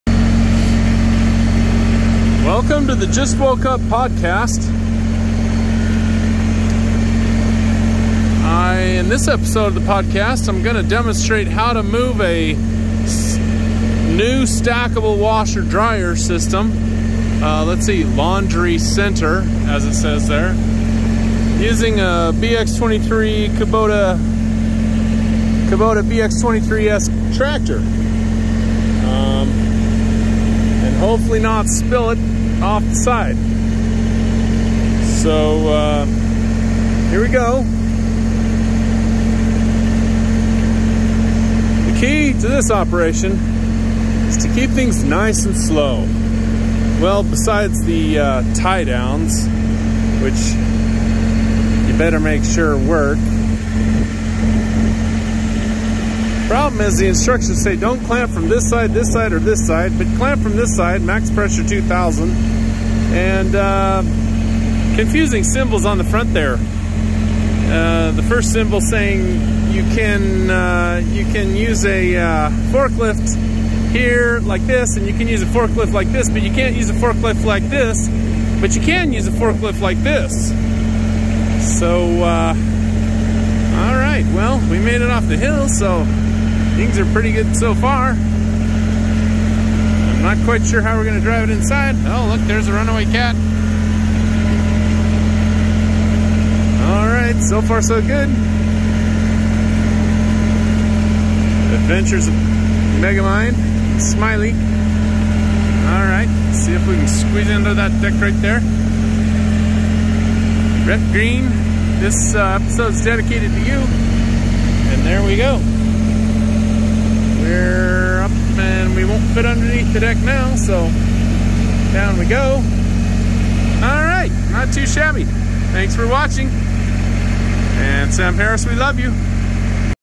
Kubota BX23s moves stackable washer dryer upright!
in this episode of the "Just woke up" podcast, megamind decides to move a stackable washer/dryer with a Kubota BX23s.  come follow along!  Sam Harris we love you.